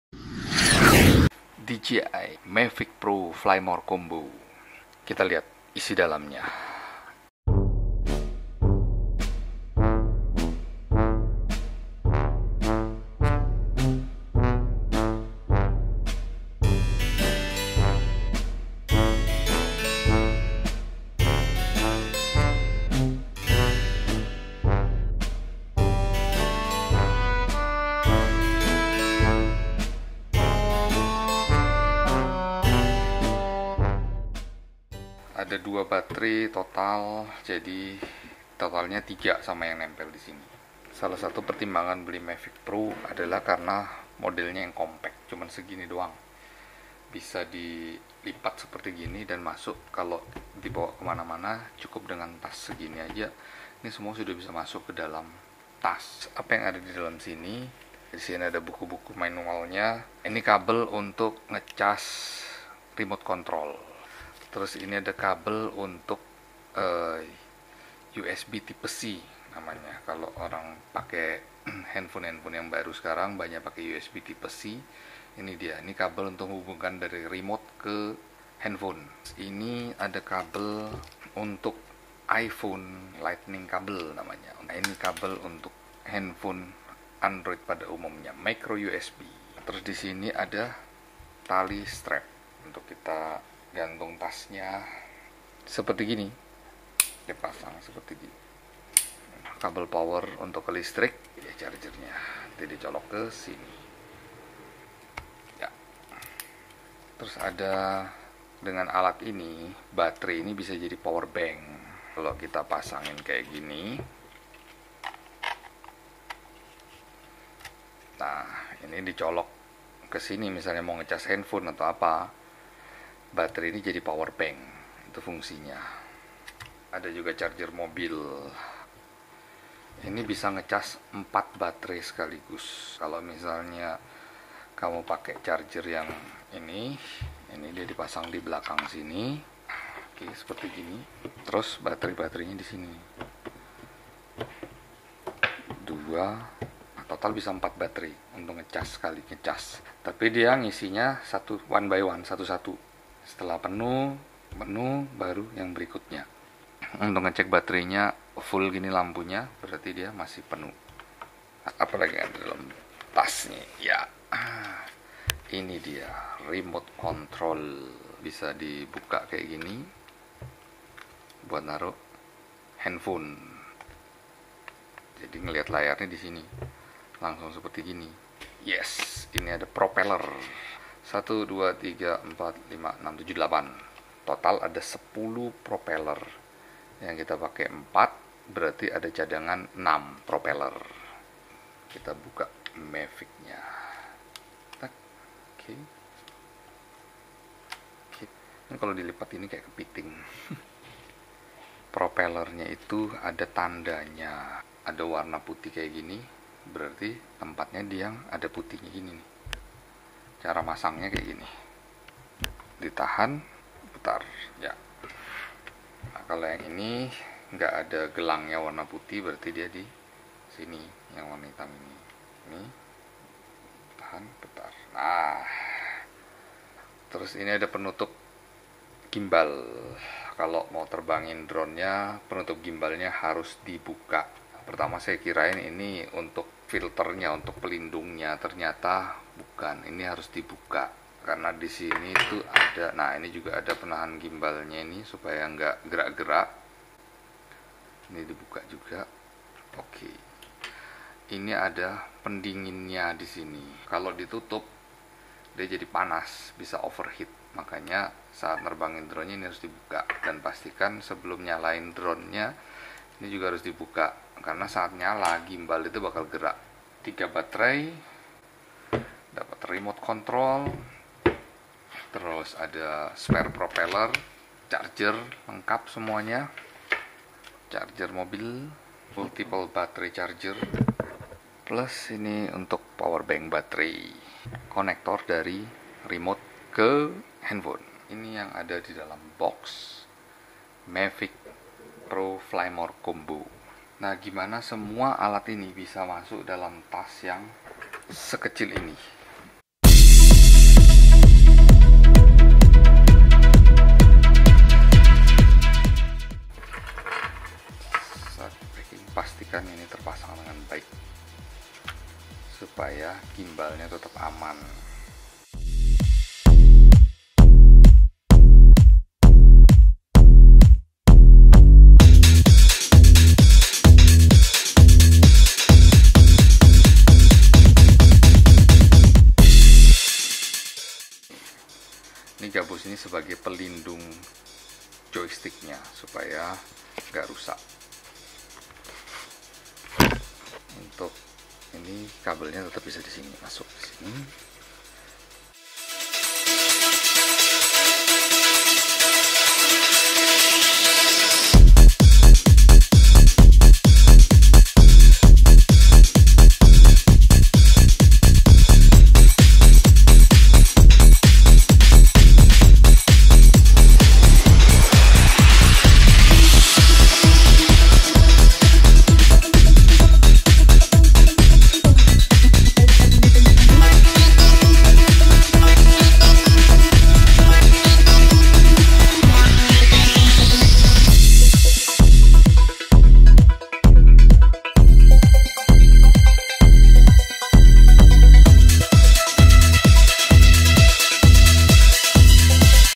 DJI Mavic Pro Fly More Combo Unboxing, Bahasa Indonesia
Apa aja yang didapat dalam paket pembelian DJI Mavic Pro Fly More Combo, bisa diliat di video ini. Salah satu alasan membeli Mavic Pro adalah karena simple dan praktis untuk dibawa kemana-mana.

Karena rekaman sebelumnya terhapus, akhirnya harus merekam lagi setelah beberapa kali pemakaian.

Jika ada pertanyaan tentang DJI Mavic Pro, silahkan isi di kolom komentar.